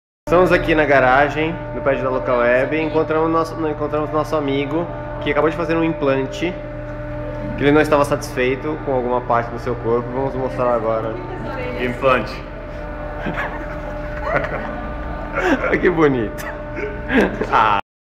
Andrew - Implante de Orelha de Mickey
Na garagem, o Andrew Andrade tem orelha de Mickey. Muito divertido! Implante de orelha de Mickey